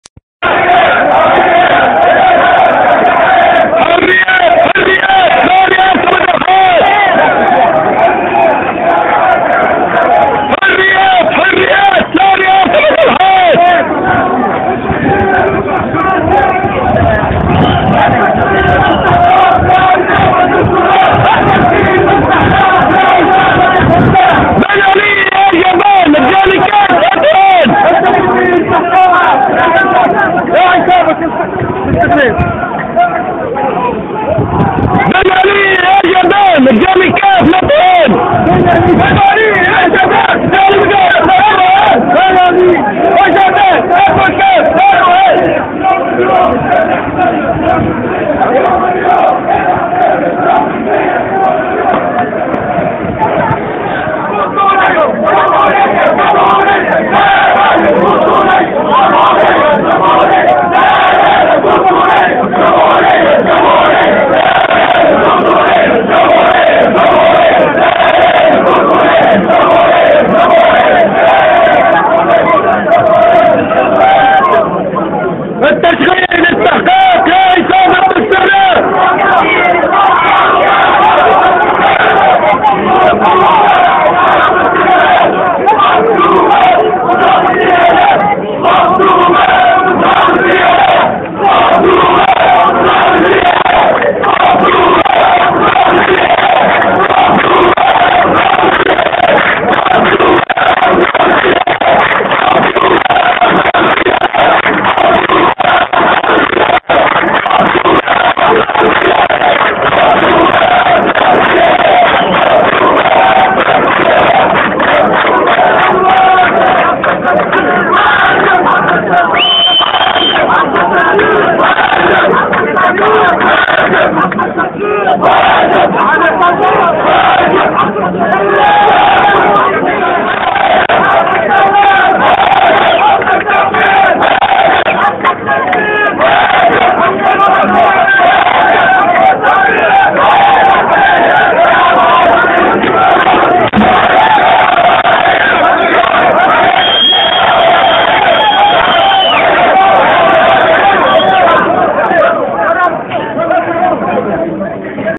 El Kef_protest_31.12.2010.mp4
La première grande manif contre Zaba au Kef, 31 dec. 2010.